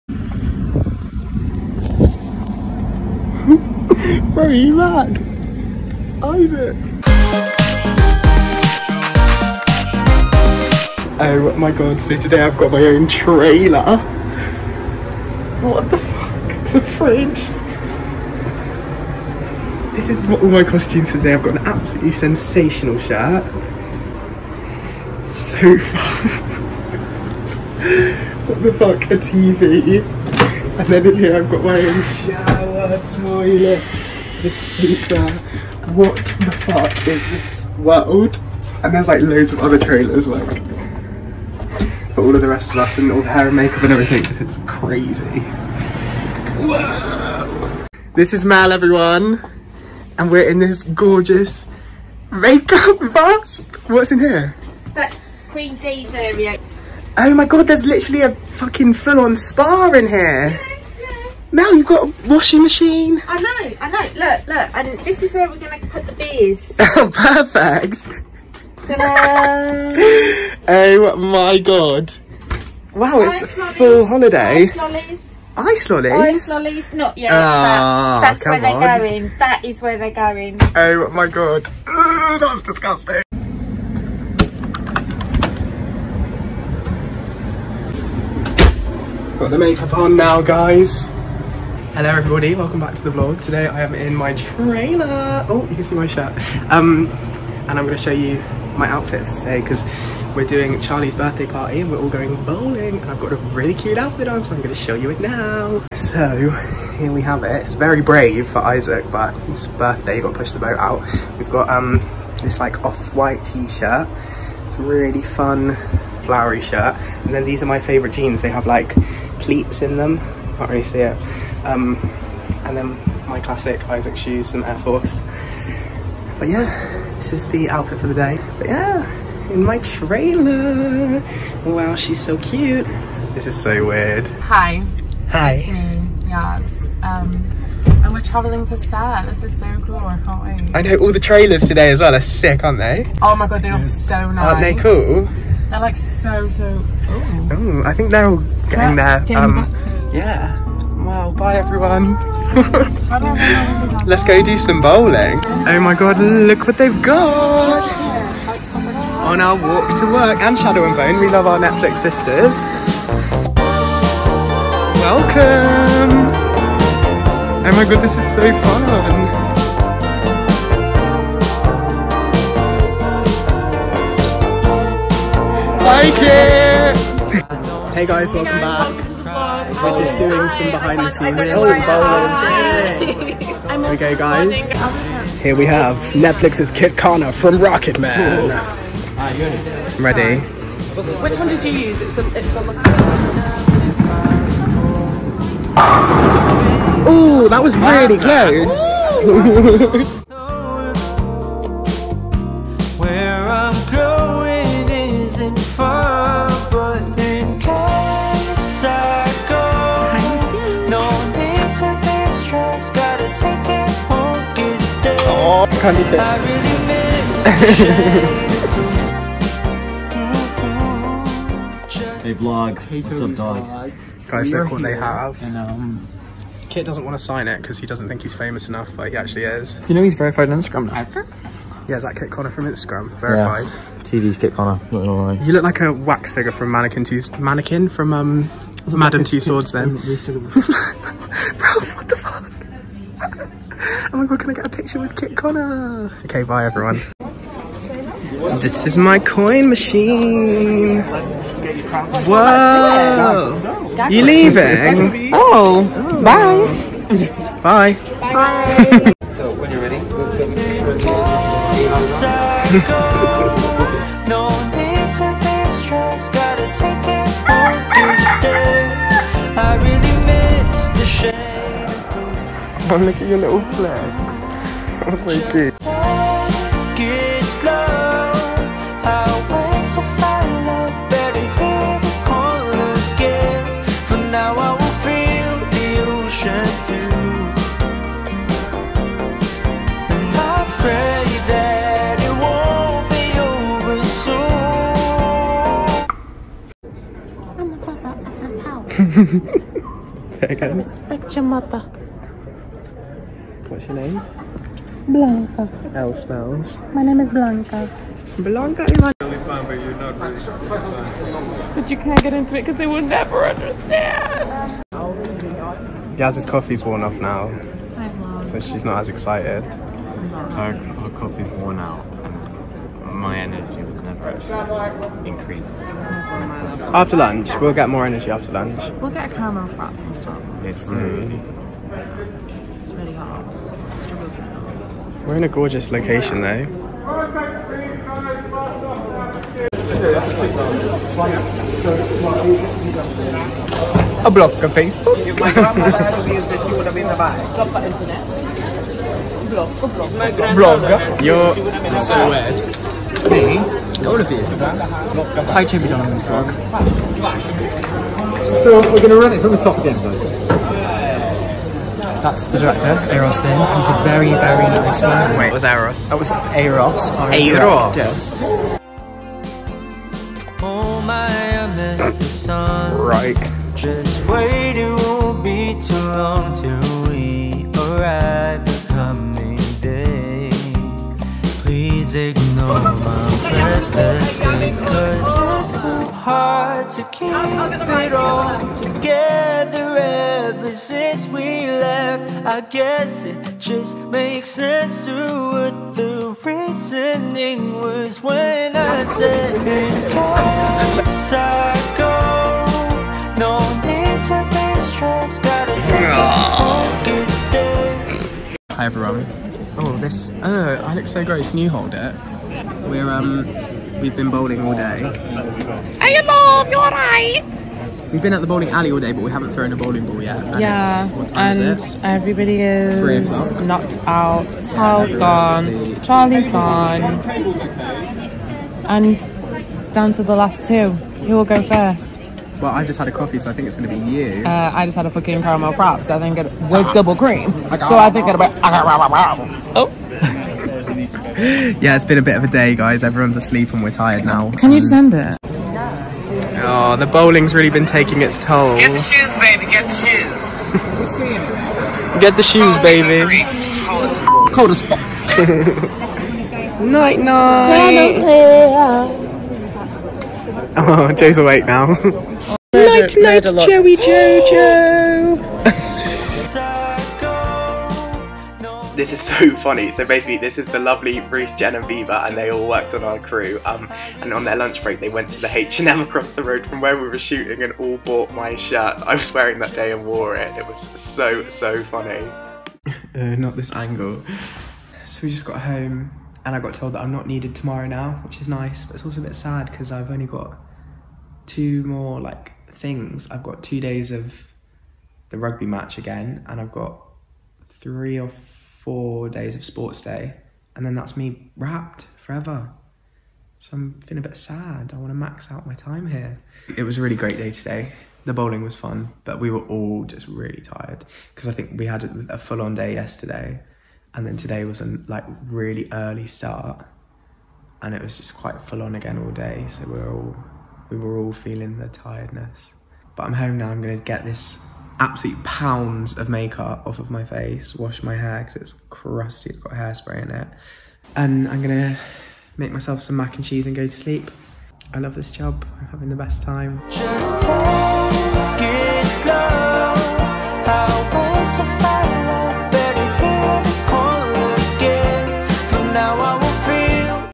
HEARTSTOPPER VLOG 5! Bowling Alley and Arcade fun! Netflix Behind the Scenes!! 🎳🍂💜
Come with us as we shoot the scenes at the Bowling Alley! In this video I get very excited about having my own trailer! We play lots of arcade games, and all get a bit tired by the end of the week! This scene in the show is one of my favourites so come behind the scenes and see how we made it!

And if you haven’t already, go and stream “Heartstopper” on Netflix NOW! 🌈🍂💜